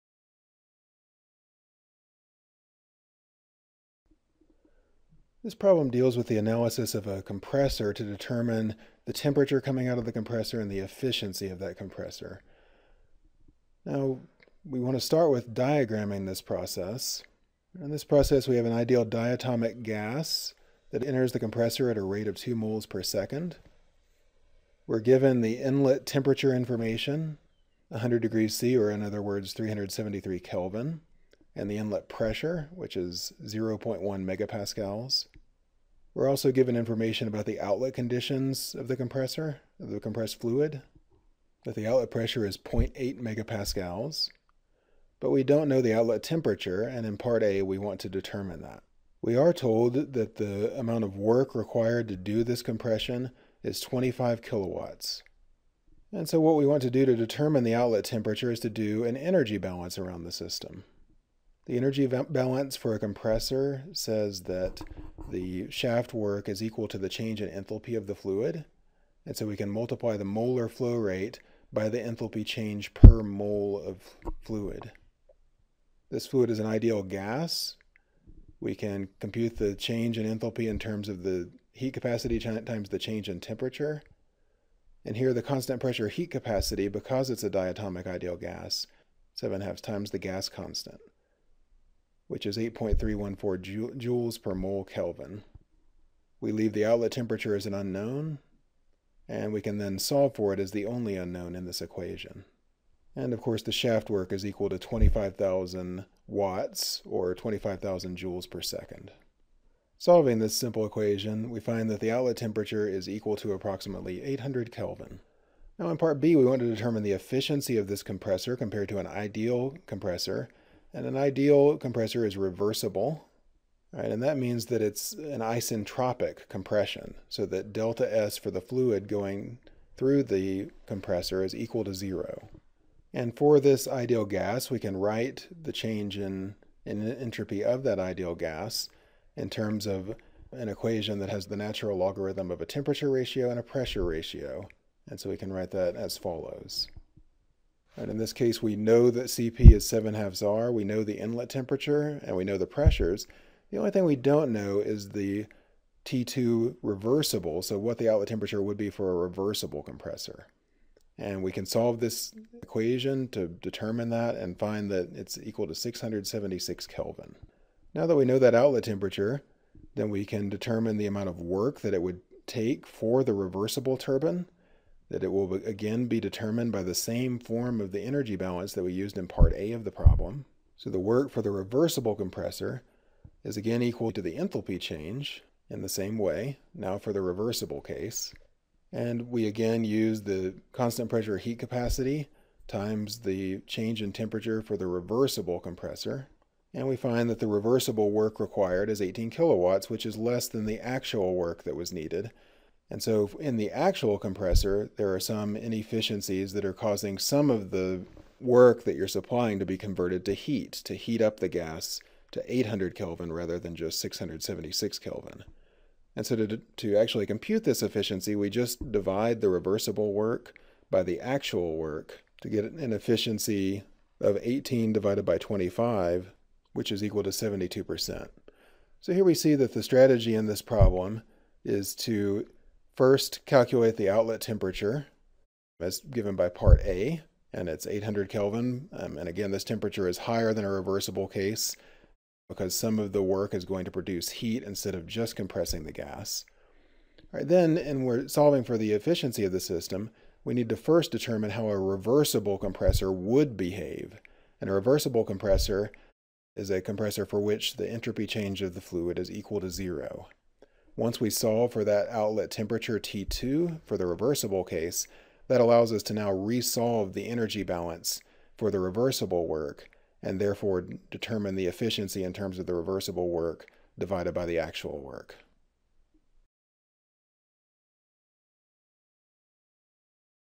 Compressor Efficiency
Calculate the efficiency of a compressor that uses an ideal gas. Made by faculty at the University of Colorado Boulder, Department of Chemical and Biological Engineering. Reviewed by faculty from other academic institutions.